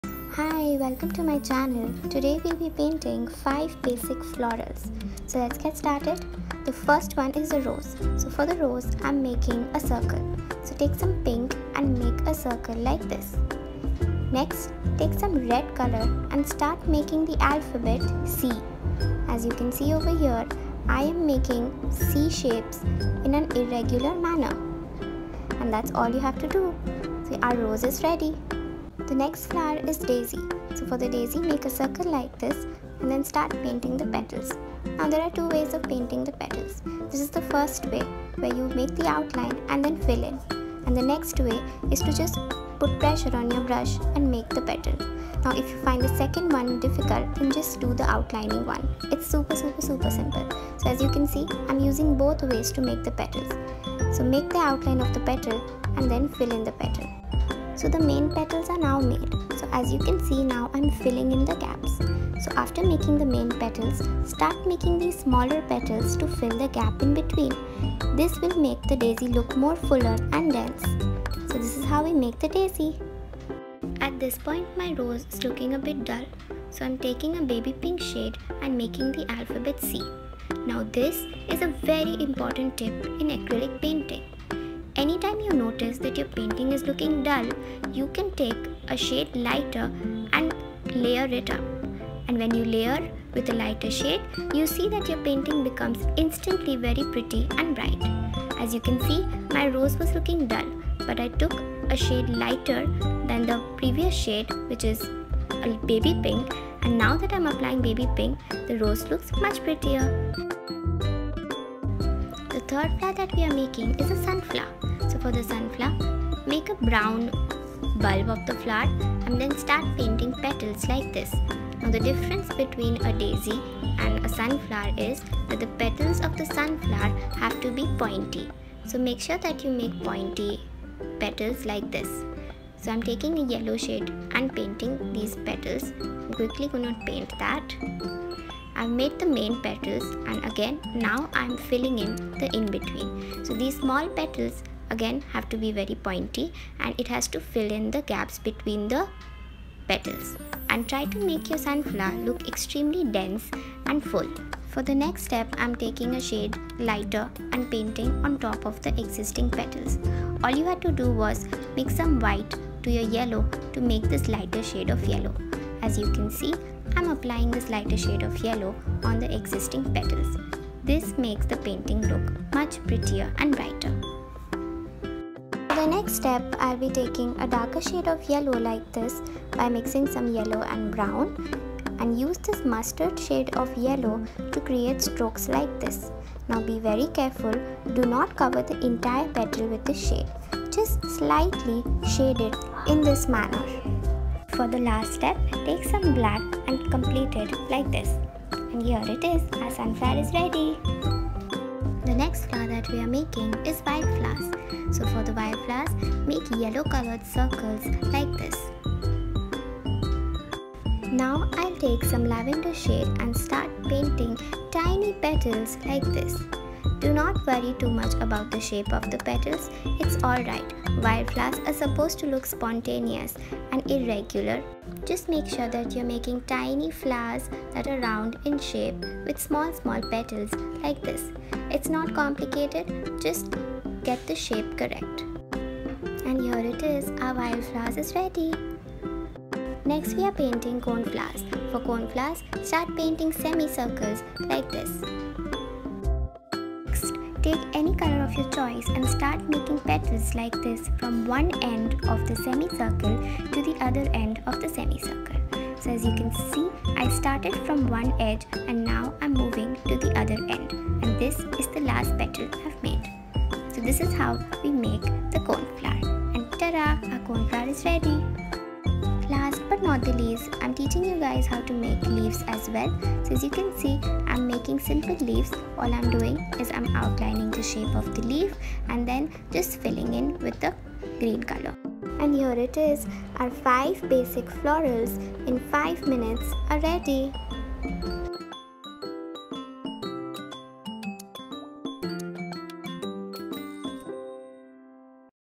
5 BASIC EASY FLORAL IN 5 MINS. Simple Acrylic floral artwork for beginners🌼
Hello lovelies welcome!!
Here are 5 basic simple acrylic florals in 5 mins.
Here is a beginners guide to easy floral painting and floral artwork. We learn to paint
1. Rose
2. Daisy
3. Sunflowers
4. Wildflowers
5. Coneflowers
Materials used
- Paper : Anupam a4 size black sketchbook
- Brush : Camlin Round brush set
For Rose, Daisy, Sunflowers and Coneflowers I have used brush no. 4 and 6 for petals
For Wildflowers I have used brush no.1 for petals
- Paints : Fevicryl Acrylic Paints
Hope you guys liked it. Have a good day.